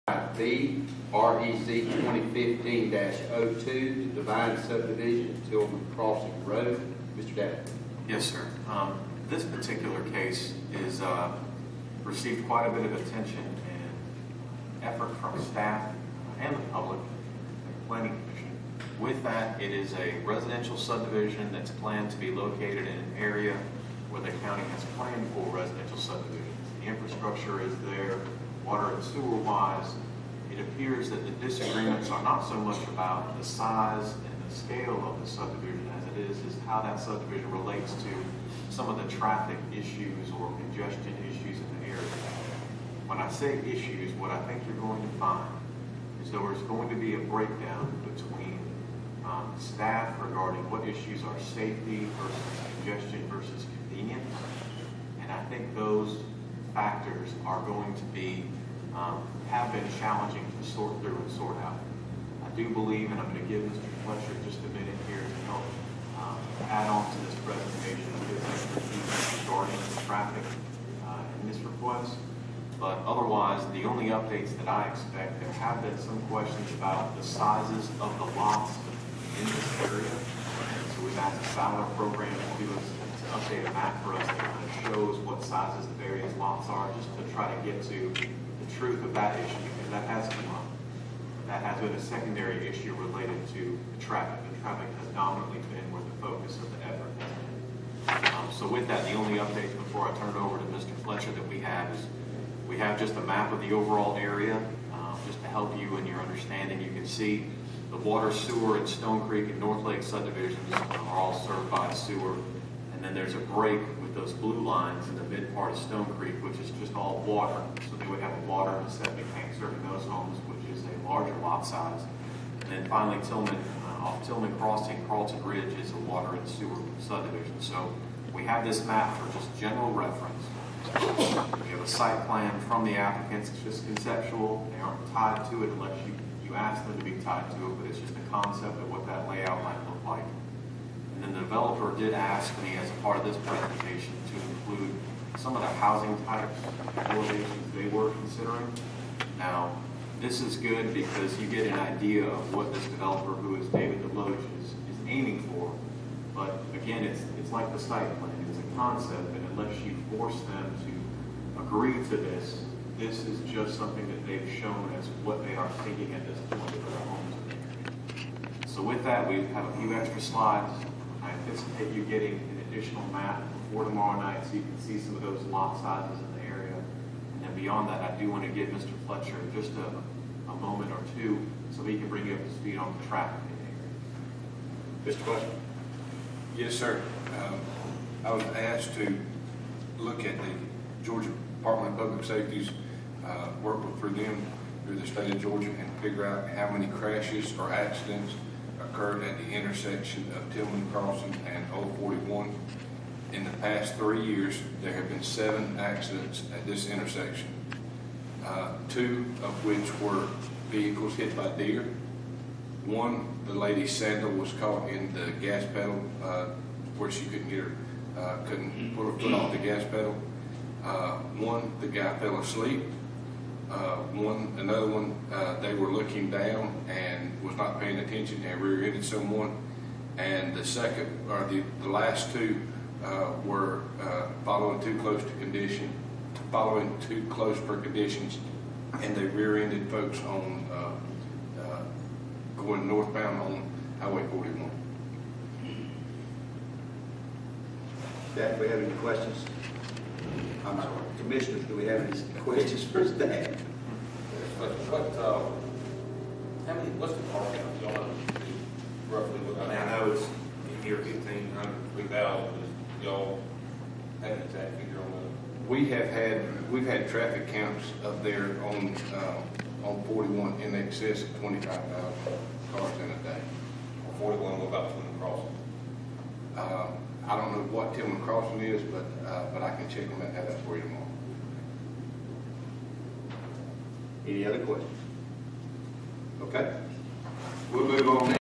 5b. REZ-2015-02 Devine Subdivision Tillman Crossing Rd, R-1 To R-10 @ LCC Work Session - 2015-02-09
TRC Recommendation: Approval with 1 Condition Regarding Access
GLPC Recommendation: Denial (7-2)